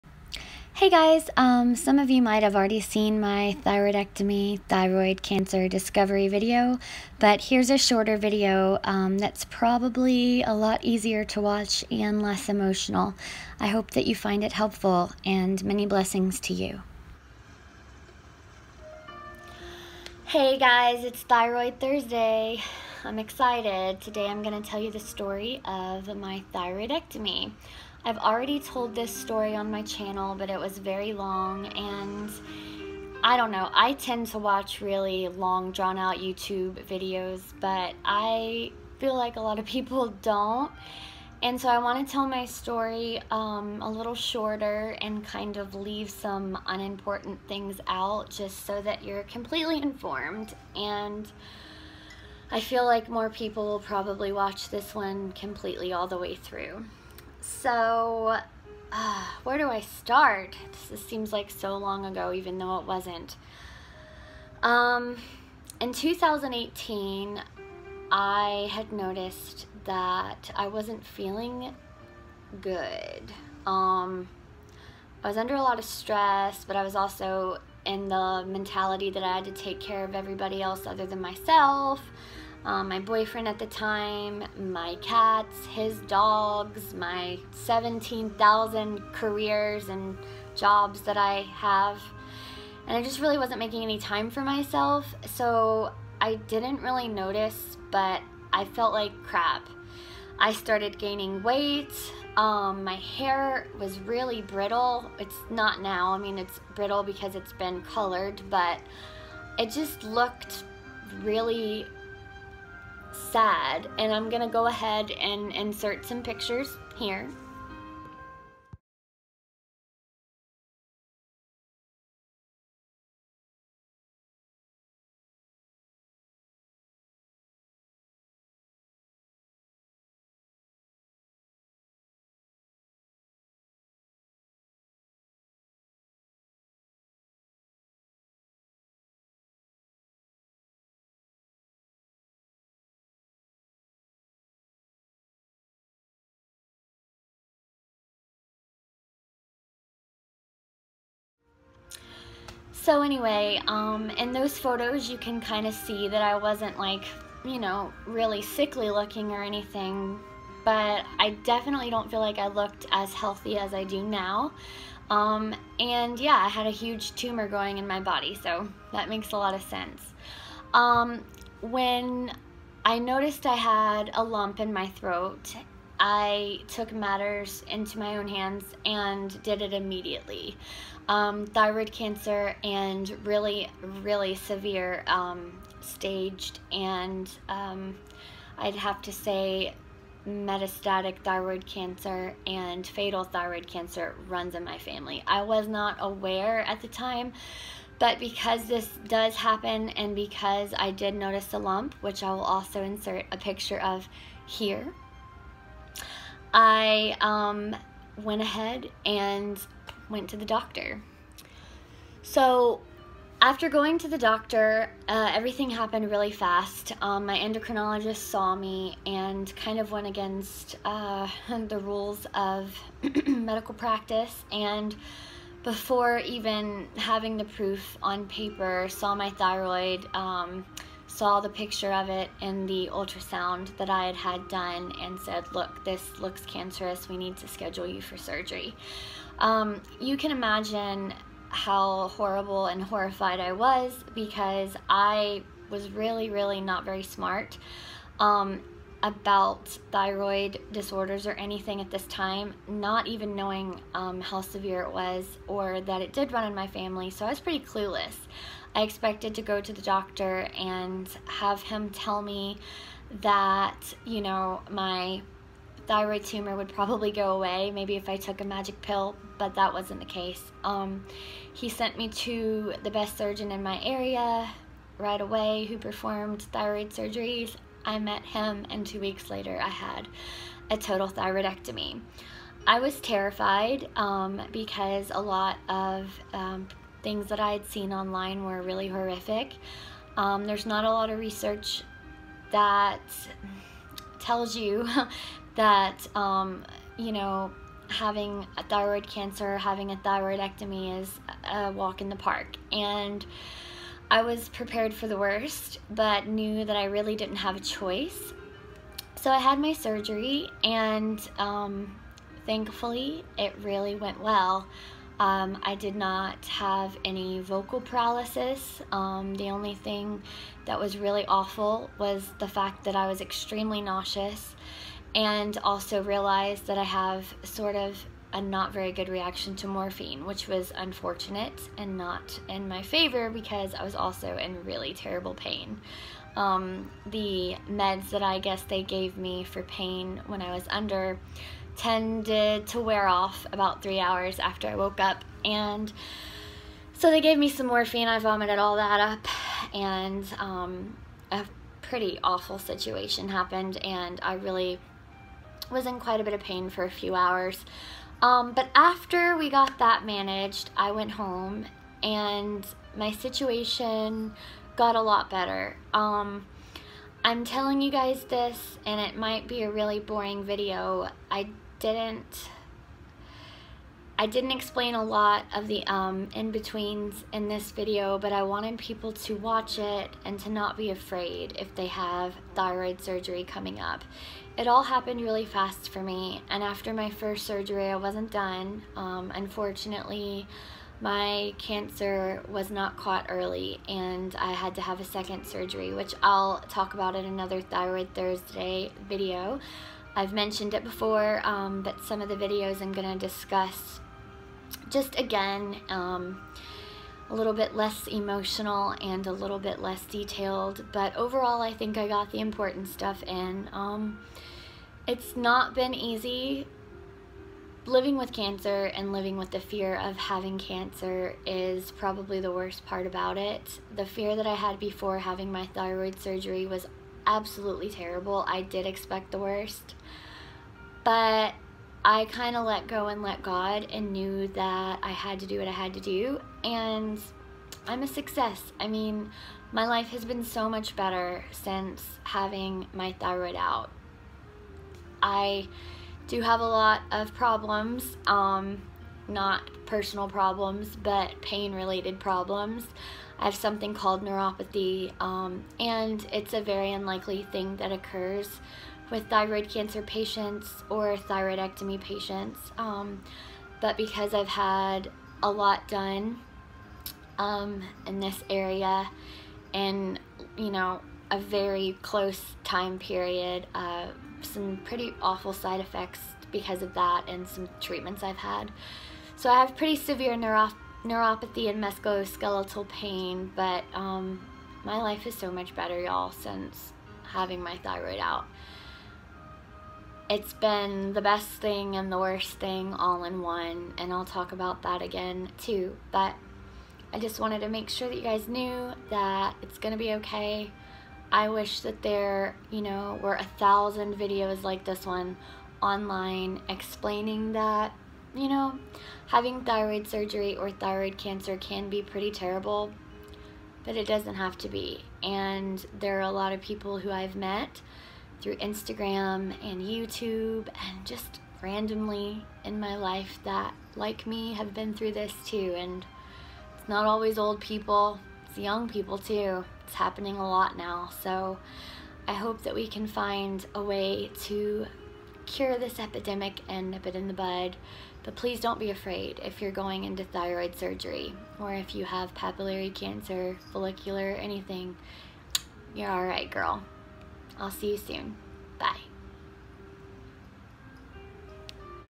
Papillary Thyroid Cancer & Total Thyroidectomy
The “Short” Version of the Beginning of my Thyroid Cancer Journey.🦋